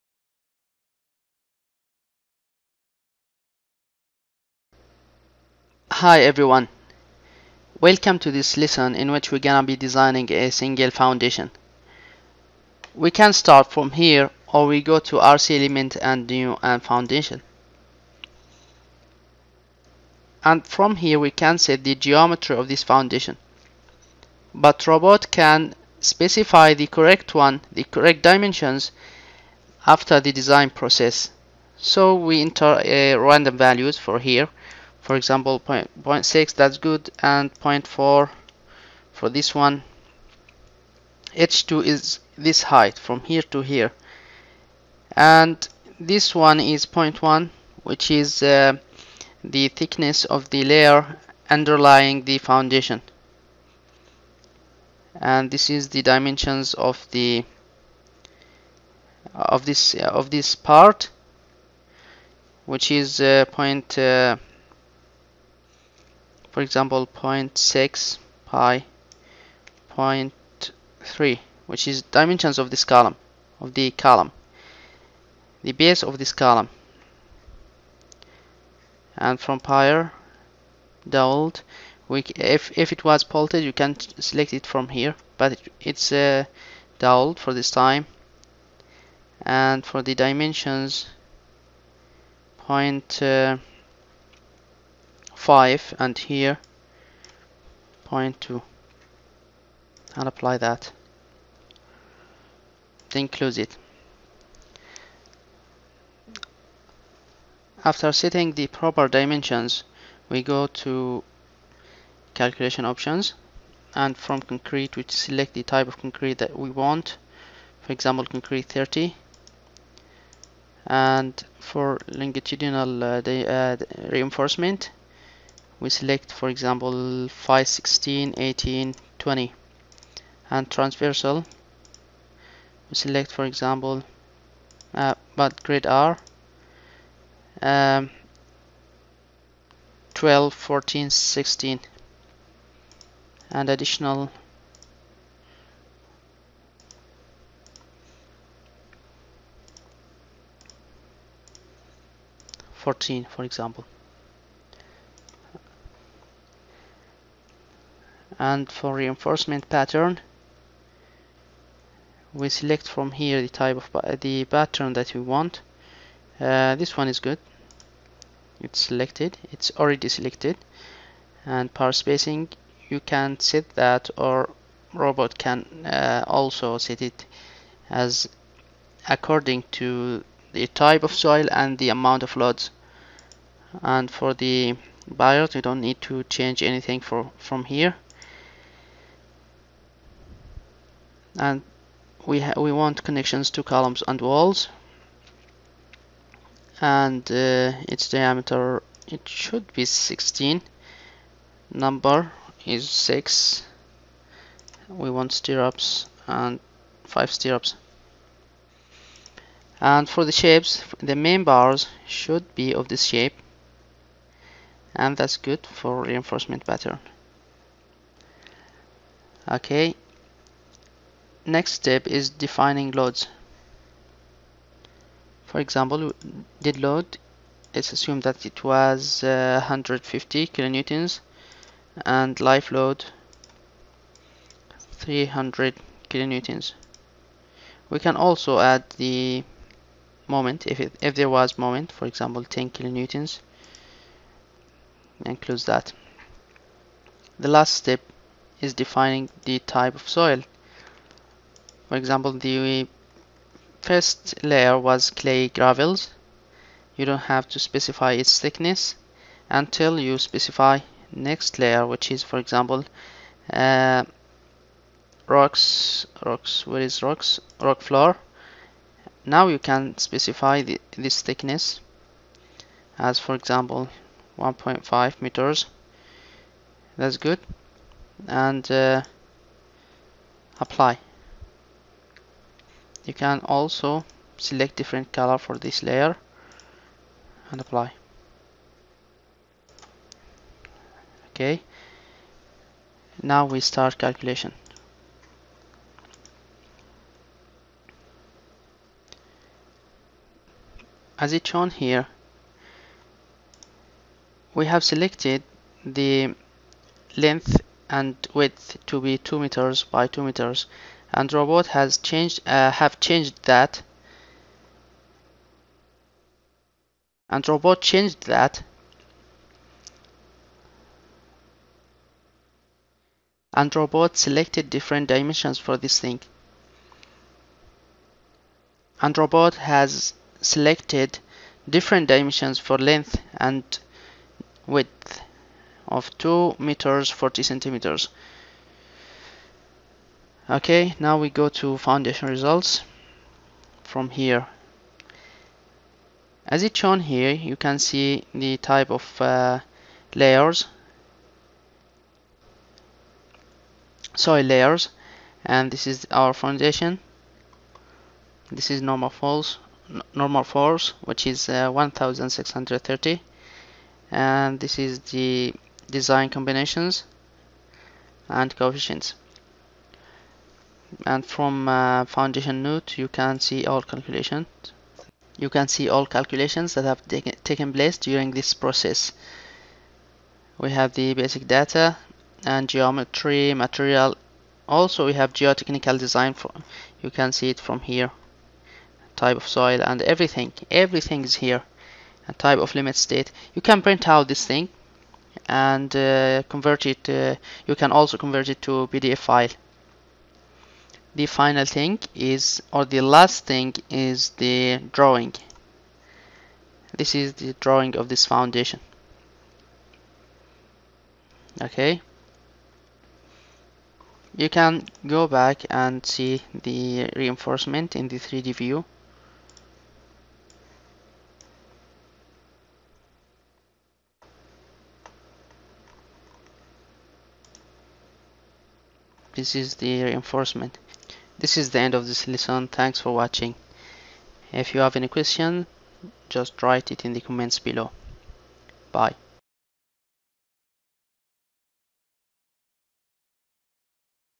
Lesson15Designing a Foundation in Autodesk Robot Structural Analysis Professional
This lesson illustrates the process of Designing an RC Foundation in autodesk robot structural analysis. Results are shown as drawings.